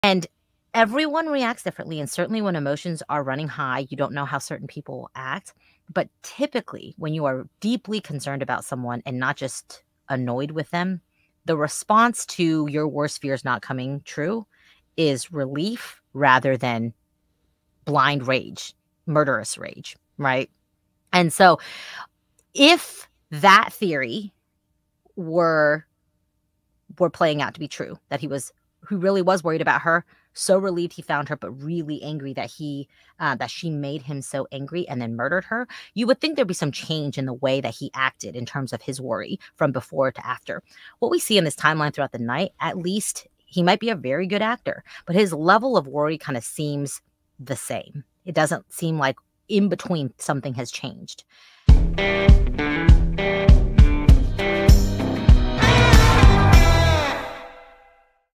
The Murder of Michelle Schofield Part 3
This week, we start our look at the evidence in this case. Does it add up? And what does Leo's behavior say about his guilt?